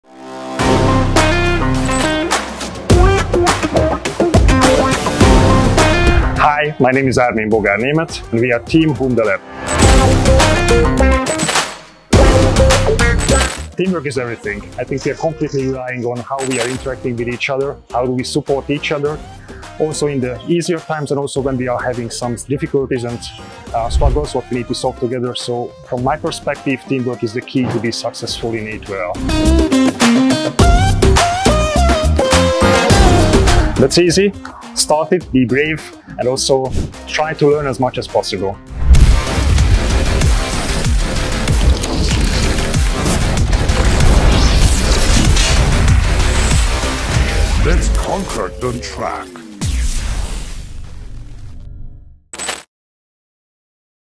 Team Humda Talks Teamwork and Innovative Strategies | A2RL
Watch Armin Boga-Nemeth show us how Team Humda harnesses teamwork and innovative strategies in A2RL’s extreme autonomous race on 27 April 2024. Their efforts and advanced simulations are key to mastering high-speed performance. See their collaboration and cutting-edge technology drive the future of transportation and mobility live at Yas Marina Circuit.